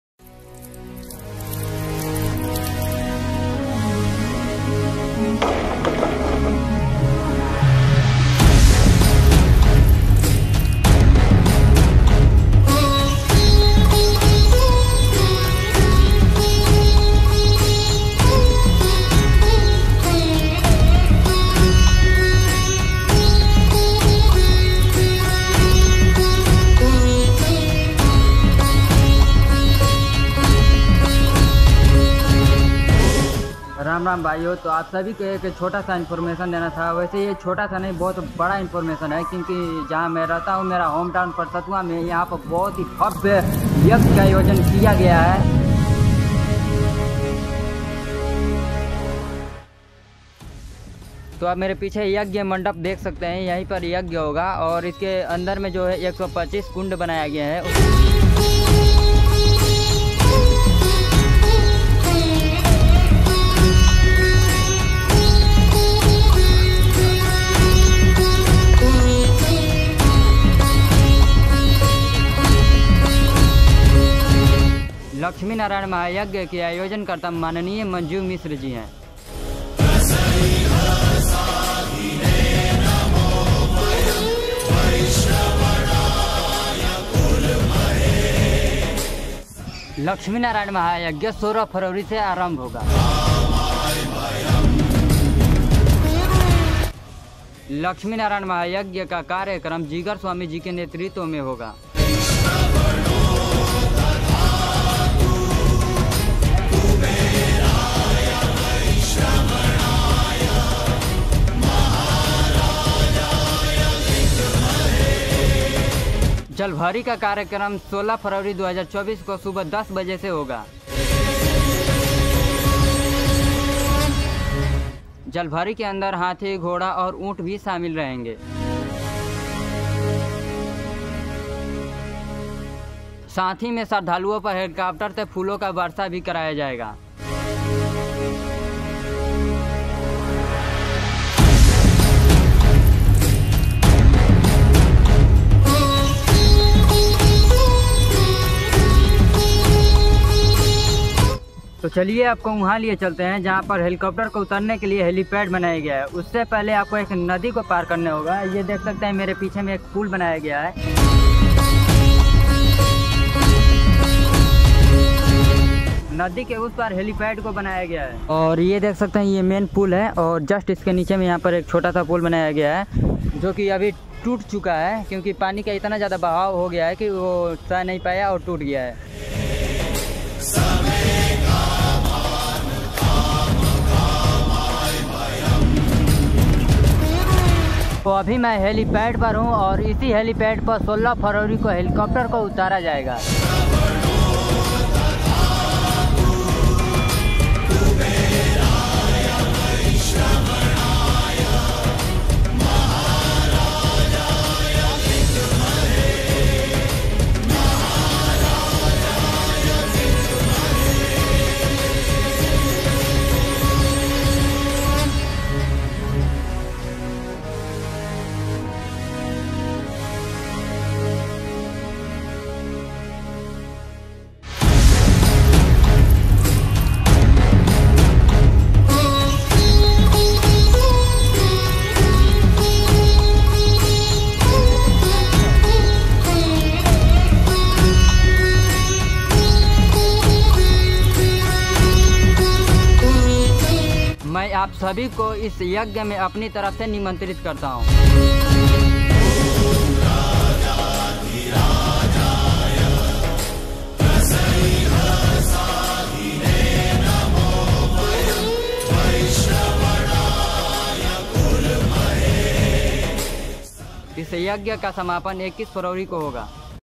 Lakshmi Narayan Mahayagya Parsathua | Jigar Swami Maha Yagya | jigar swami ji ka katha
लाच्छामि नारायण  महायज्ञ का आयोजन करता | श्री गिरीश नारायन मिश्रा  महाविद्यालय | के सच्चीव माननीय माँजीव मिश्र हैं |

लाच्छामि नारायण  महायज्ञ:   16 Fab 2024 से  आराभ  होगा |
यज्ञा का कार्यक्रम,  जिगर सोवमी के नेतिरित्व मे होगा |

जलभारी का कार्यक्रम 16 Fab  सुबह 10 बजे से बासाही नदी के लिए आरभ होगा |
जलभारी मे हाथि, घोड़ा, और  उट  भि शामिल रहेंगे |
साथहि मे  Helicopters से सरधलुओ पर. फूलो का बारशा भी कराया जौएगा

मै आप सभी को इश महायज्ञ मे अमानतिरित करता हु ।

जय श्री लच्छिमी नारायन 🙏